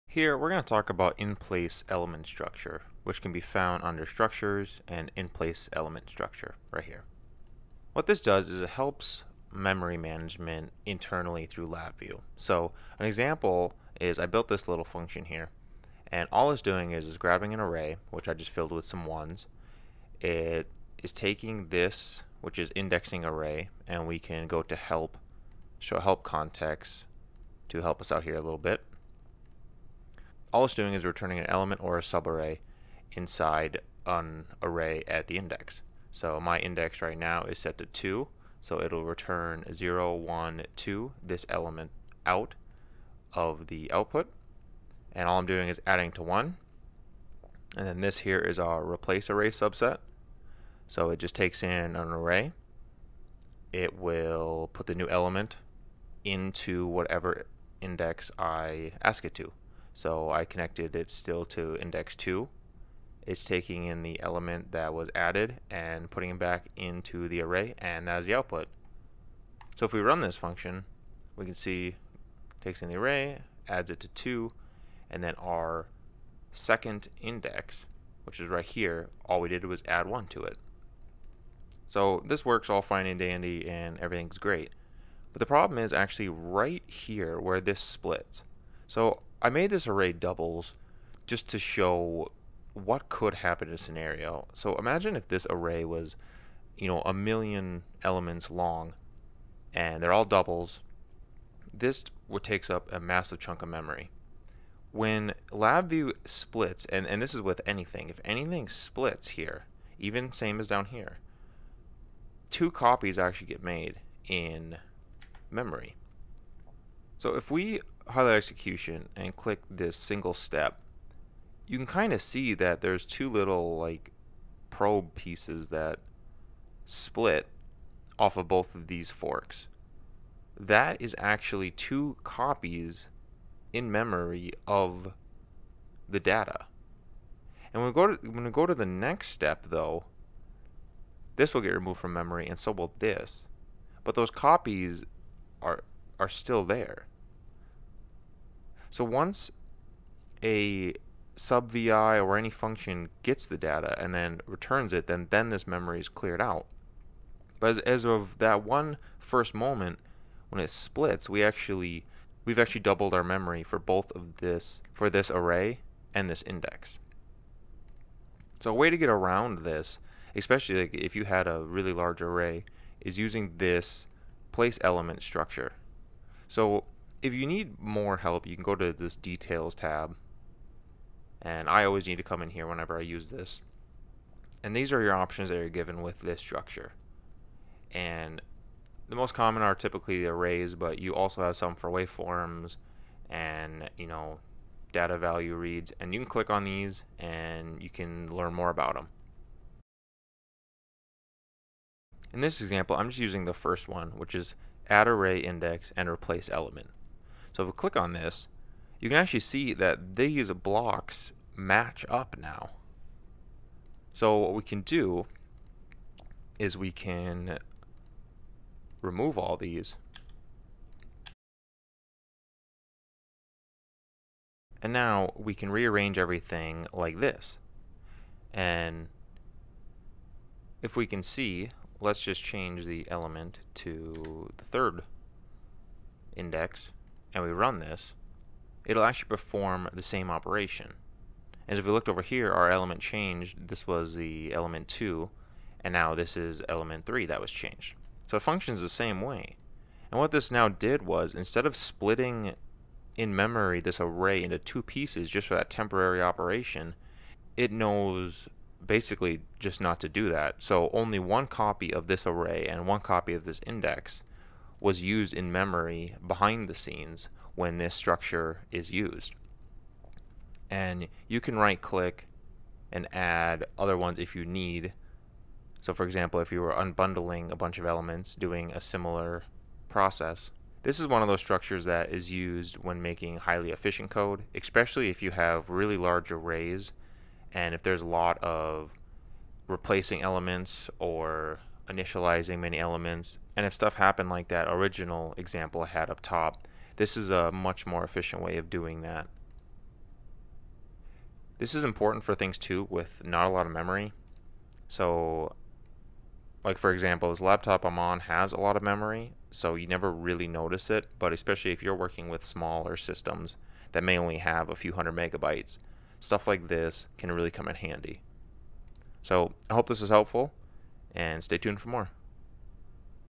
LabVIEW Place Element Structure - Basics
This video covers the basics of a Place Element Structure in LabVIEW. I plan to continue making videos of all LabVIEW topics. The videos will stay short and get right to the point!